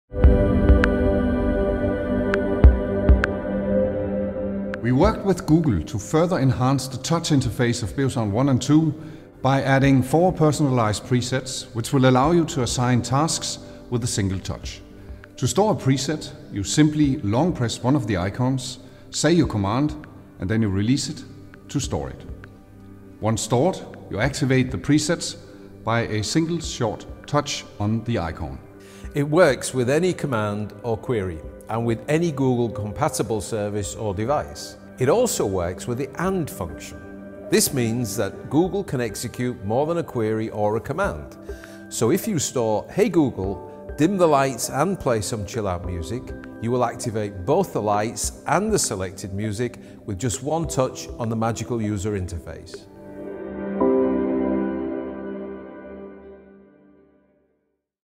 Beosound 1 & 2: Assigning Presets
Find out how to assign presets for your Beosound 1 or Beosound 2.